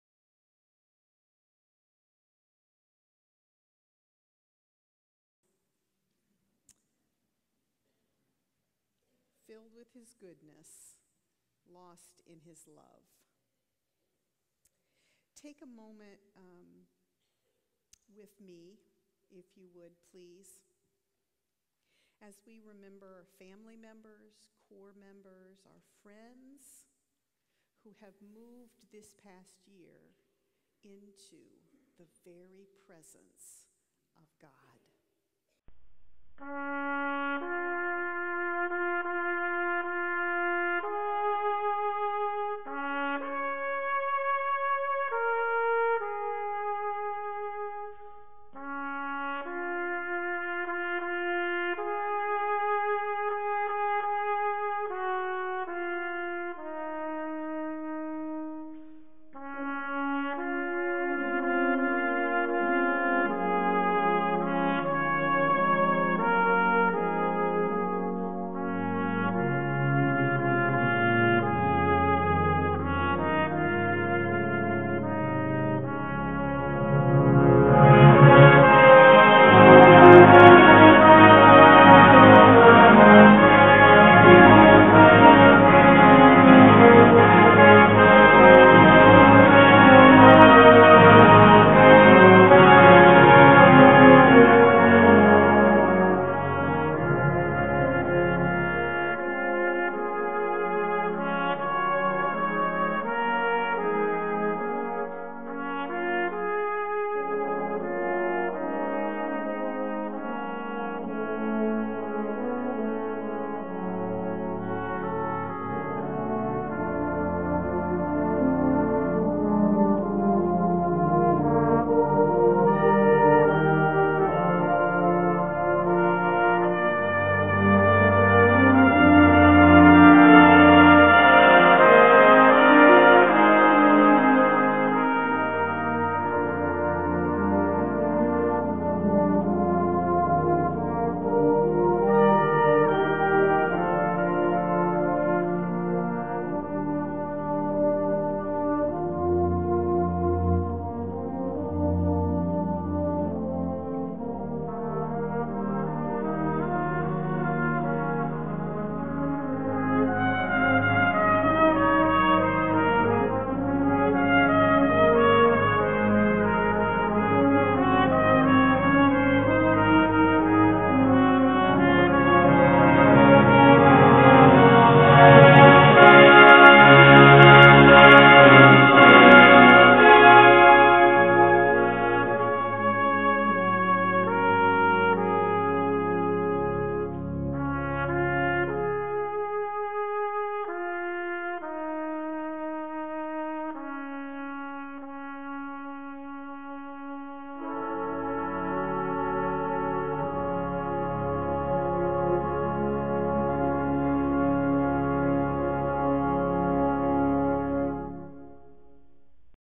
We Remember Slideshow
Major Pamala Morris introduces the 2018 Promoted to Glory Slideshow during The Salvation Army Clearwater Citadel Corps Holiness Meeting on 1/6/19